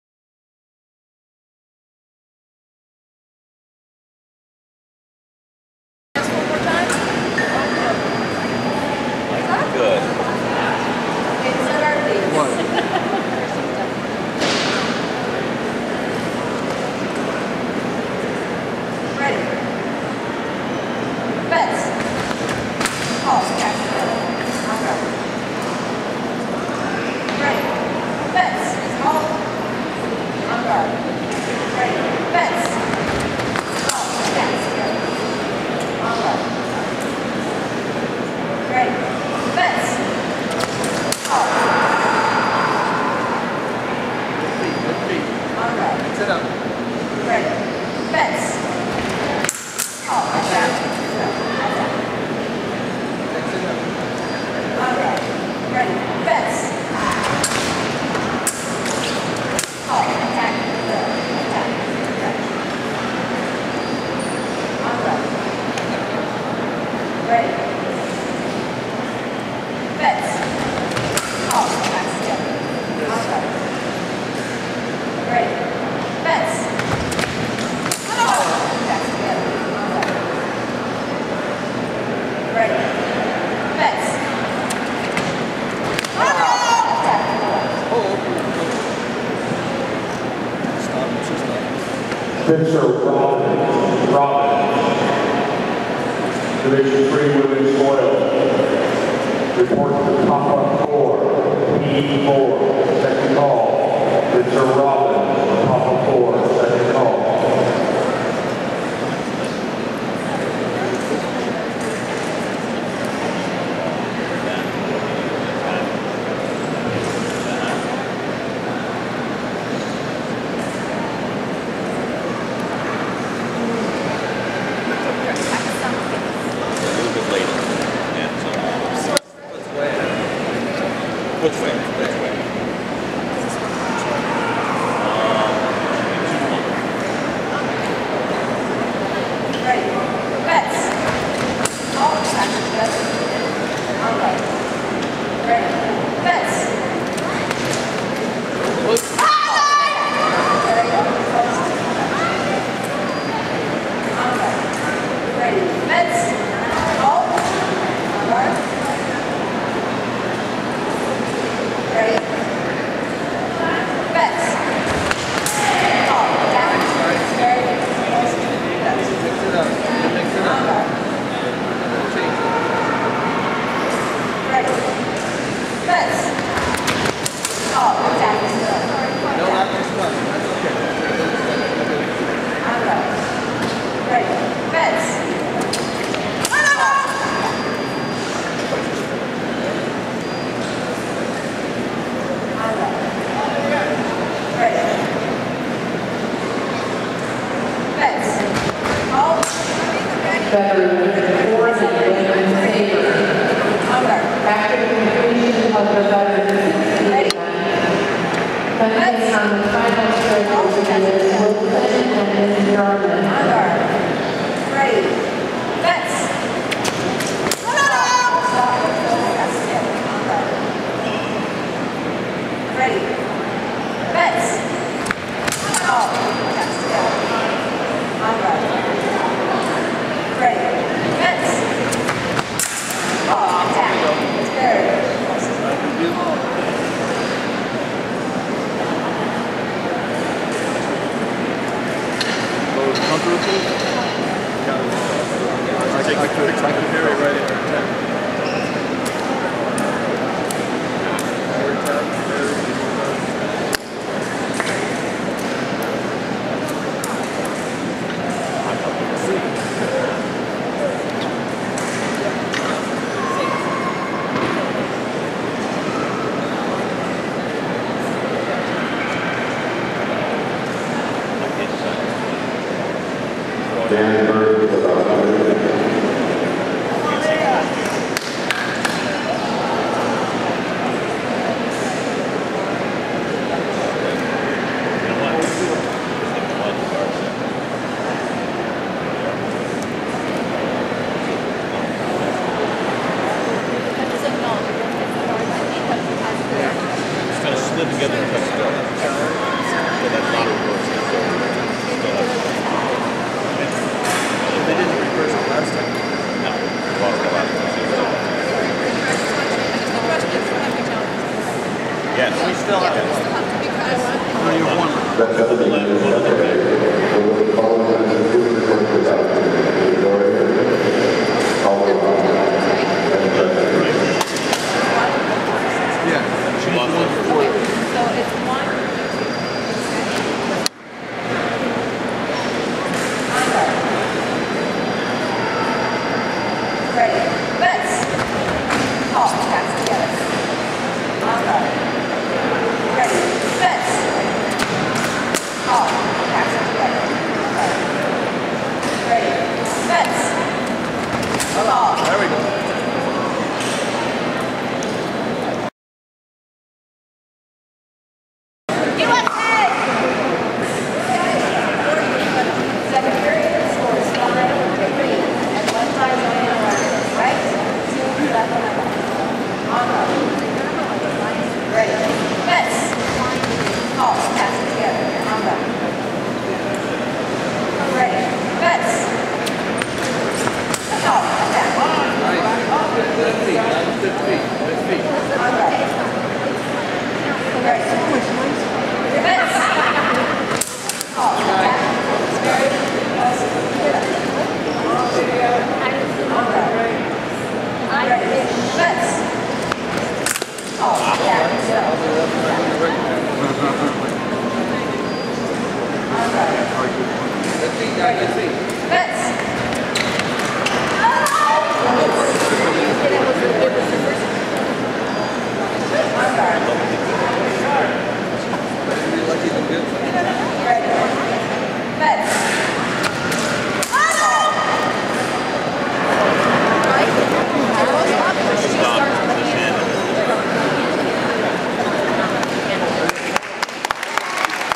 Veteran 50-59 Women's Saber Gold Medal Final - 2014 USA Fencing National Championships
Lydia Mazorol vs Chaz Smith